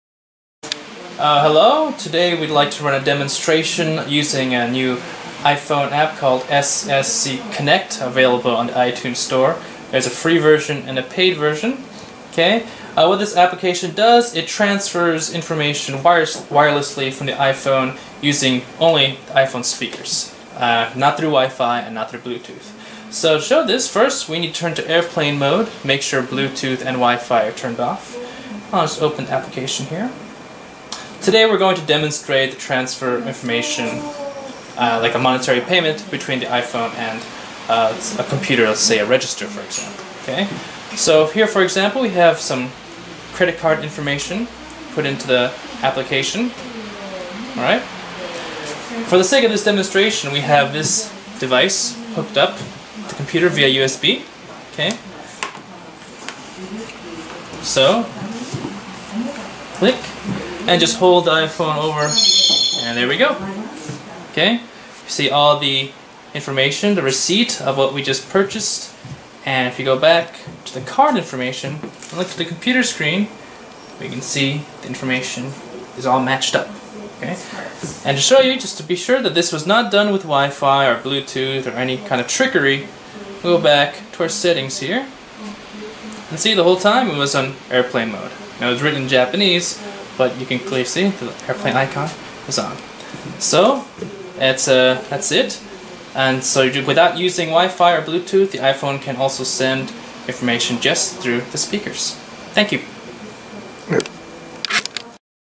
"ultrasonic communication" Electronic payment demonstration
This video is a demonstration of electronic payment using an application that was installed on the iPhone and the two-way ultrasonic communication module.

In order to clarify the communication is only ultrasonic communication, ultrasonic data communication is performed after the state in which the mobile data communication stops the iPhone.
Also, turn off bluetooth and WiFi.
(Of course, even in a state where you can use the mobile data communications, Bluetooth, WiFi, ultrasonic communication is possible.)

The information on a receipt is transmitted to iPhone from the personal computer side, card information is transmitted to a personal computer from iPhone, and it is displayed on a display.